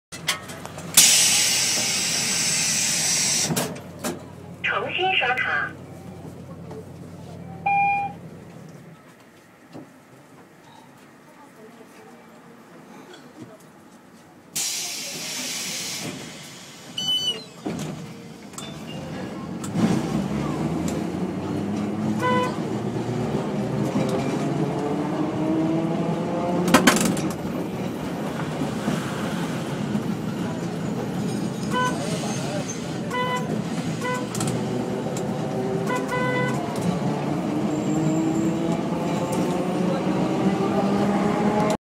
#201 Trolley in Dalian
it took me over 75 minutes to take the only two trolleys in Dalian from the office to my service apartment. no traffic, 25 minute taxi ride.